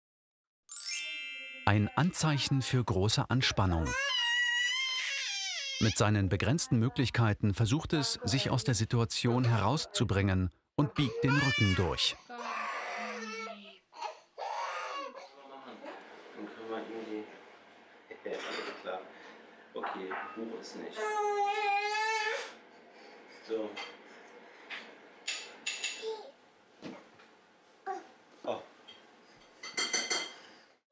Rücken biegen
Das kleine Kind biegt den Rücken durch. Im Kanal sind weitere kurze Filme darüber zu sehen, was Babys mit ihrem Rücken so anstellen.